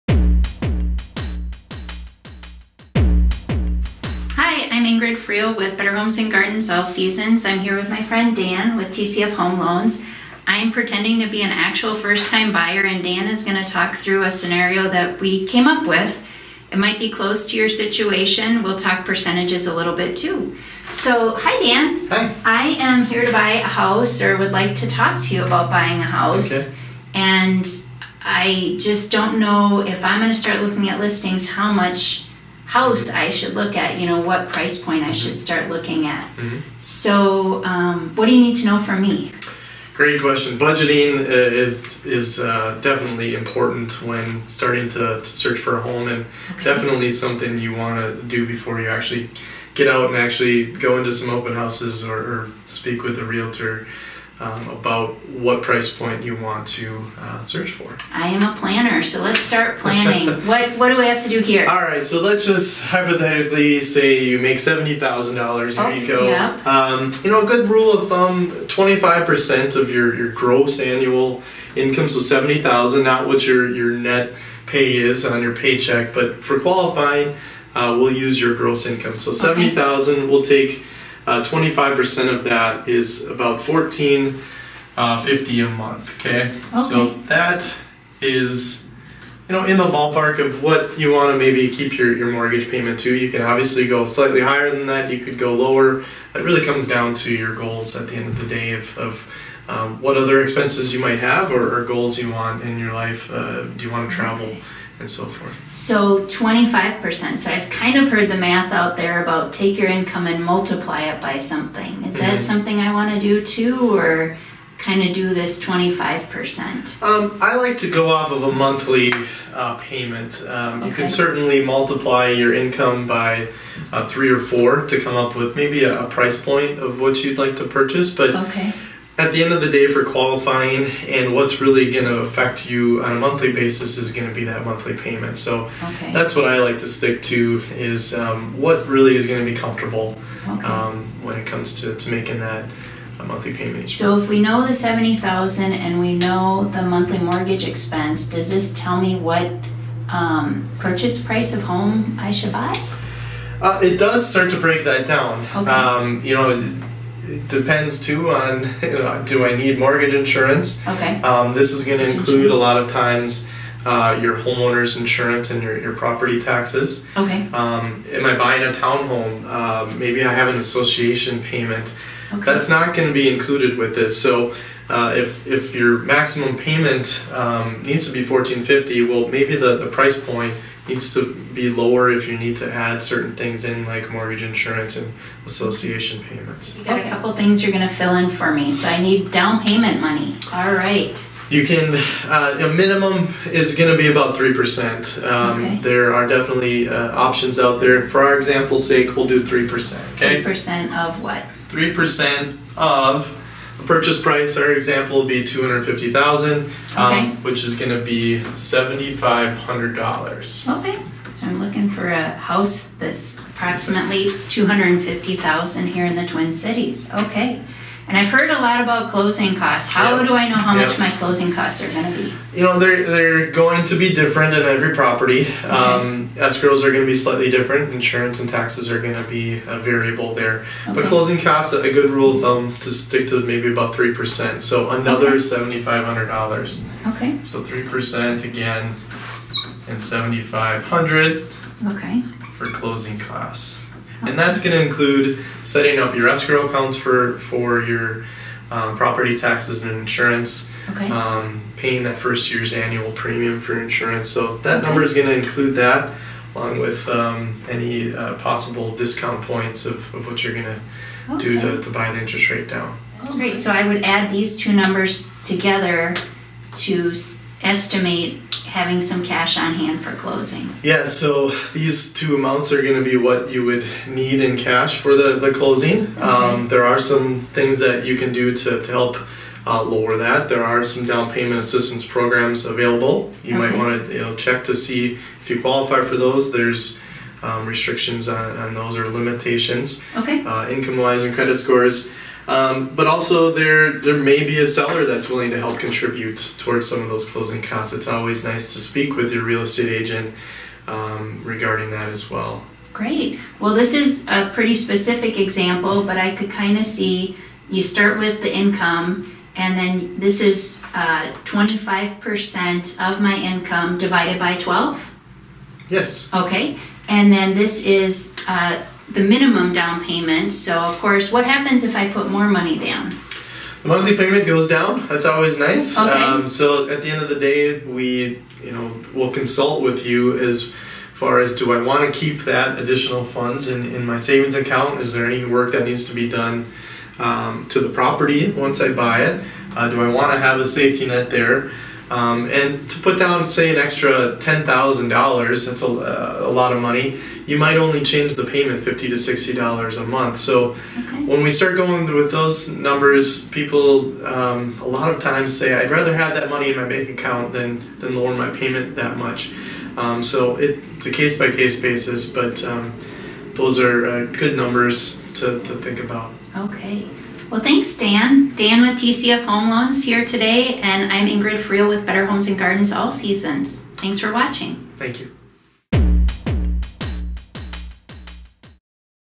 Lender Talks Thru Buyer Example
Hi, I'm Ingrid Friel with Better Homes and Gardens All Seasons. I'm here with my friend Dan Eveland with TCF home loans. I am pretending to be an actual first-time buyer and Dan is going to talk through a scenario that we came up with.  It might be close to your situation. We'll talk percentages a little bit too.  So, hi Dan I am here to buy a house or would like to talk to you about buying a house and I just don't know if I'm going to start looking at listings how much house I should look at.  You know what price point I should start looking at so what do you need to know for me?  Great question, budgeting is is definitely important when starting to search for a home. And definitely something you want to do before you actually get out and actually go into some open houses. Or speak with the realtor about what price point you want to search for. I am a planner so let's start planning. Right what do I have to do here all right so let's just hypothetically say you make $70,000. A good rule of thumb 25 percent of your gross annual incomes of 70,000. Not what your net pay is on your paycheck but for qualifying will use your gross income so $70K will take 25% of that.  It’s about $1,450 a month. Okay so that is you know in the ballpark of what you want to maybe keep your mortgage payment.  You can obviously go slightly higher than that you could go lower it really comes down to your goals at the end of the day. Of what other expenses you might have or goals you want. If you want to travel and so forth.  So 25%.  I've kind of heard the math out there about take your income and multiply it by something. Is that something I want to do? Or do this 25%?  I like to go off of a monthly payment. You can certainly multiply your income by three or four to come up with maybe a price point of what you'd like to purchase.  But  at the end of the day for qualifying and what's really going to affect you a monthly basis is gonna be that monthly payment. So that’s what I like to stick to. It's what really is gonna be comfortable when it comes to making that. So if we know the $70,000 and we know the monthly mortgage expense, does this tell me what purchase price of home I should buy? It does start to break that down. You know it depends too on... do I need mortgage insurance? This is going to include a lot of times you're homeowners insurance and your property taxes. Am I buying a townhome? Maybe I have an association payment. It’s not going to be included with this. So if your maximum payment needs to be $1,450 well maybe the price point needs to be lower. If you need to add certain things in like mortgage insurance and Association payments. Well there are a couple things you are going to fill in for me. So I need down payment money. All right. A minimum it's going to be about three percent. There are definitely options out there for our example sake we'll do three percent okay. Three percent of the purchase price for example. Be $250,000 which is
gonna be $7,500 okay I'm looking for a house this approximately 250,000.  Okay and I've heard a lot about closing costs how do I know how much my closing costs are gonna be? You know they're going to be different than every property. Escrows are going to be slightly
different insurance and taxes are going to be a variable.  A good rule of thumb to stick to may be about 3%.  So another $7,500 okay. So 3% again and 7,500 okay for closing
costs and that's going to include setting up your escrow accounts for your property taxes and insurance paying the first year's annual premium for insurance.  So that number is going to include that along with any possible discount points of what you're gonna do to buy an interest rate down.  That’s great.  So I would add these two numbers
together to estimate having some cash on hand for closing? Yes! So these two amounts are going to be what you would need in cash for the closing. There are some things that you can do to help lower that.

So it's a case-by-case basis. But those are good numbers to think
about.  Okay.  Well thanks Dan!

Dan Eveland with TCF home loans here today. And
I'm Ingrid Friel with Better Homes and